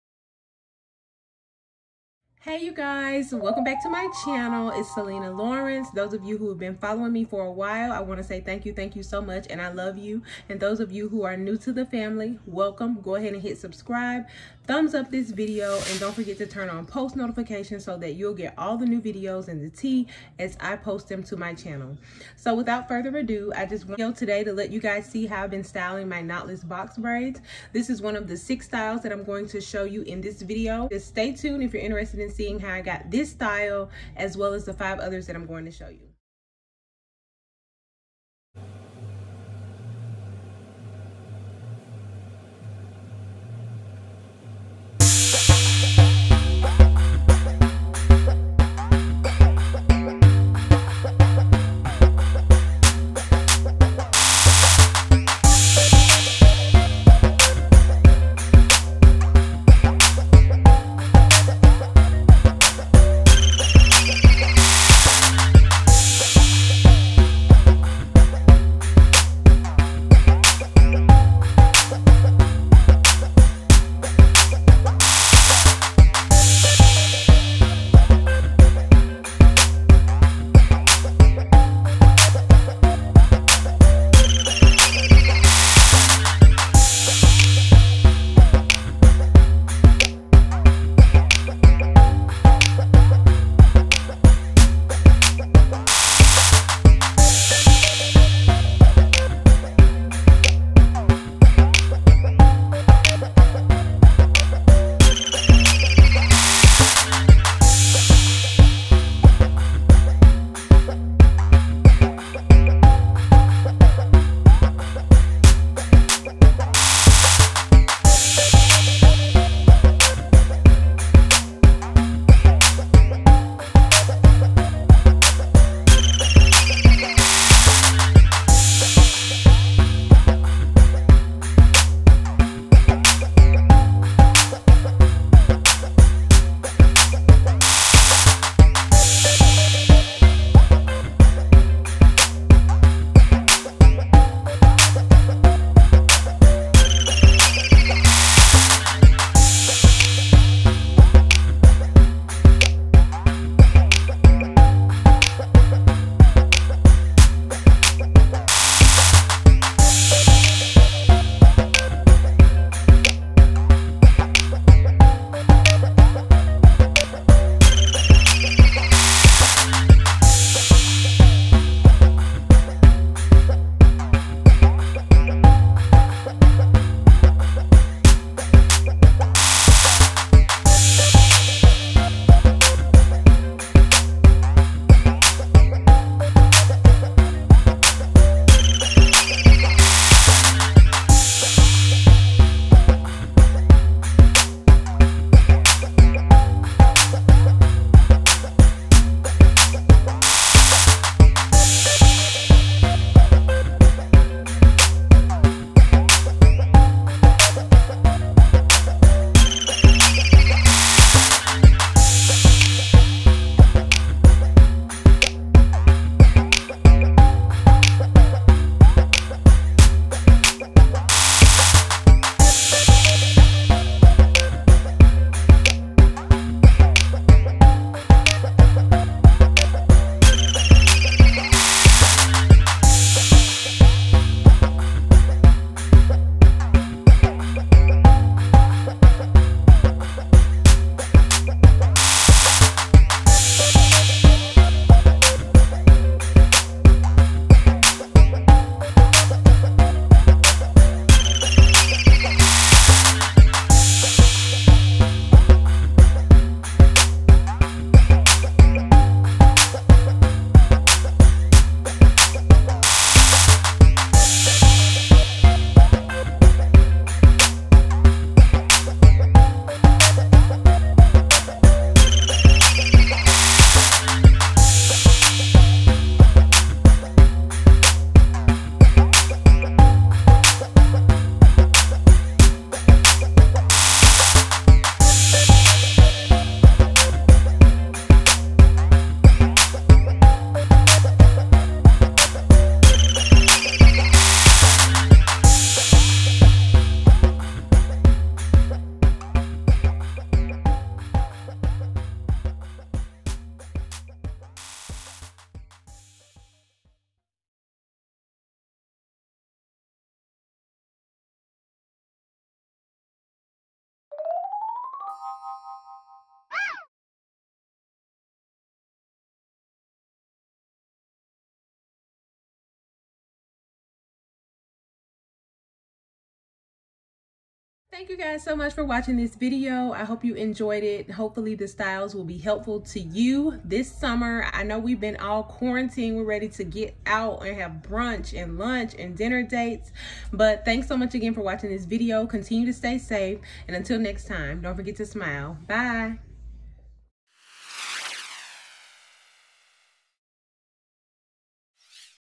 6 WAYS TO STYLE KNOTLESS BRAIDS | GODDESS PONY + HALO TWIST
This video features 6 different styles for knotless braids. Hopefully you’ll find something suitable for all occasions!!!